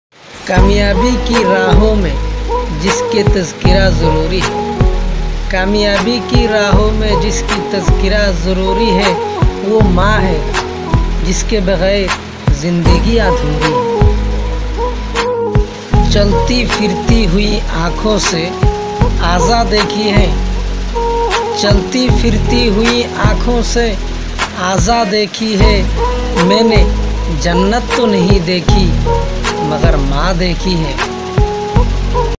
Maa ke baare me do sher মা জনন দুই লাইন
jiske maa nehi he usko posho maa kesi hoti he aur jiska maa rehte hoye bhi izzat nehi karte usko pocho jahannam keisi hoti he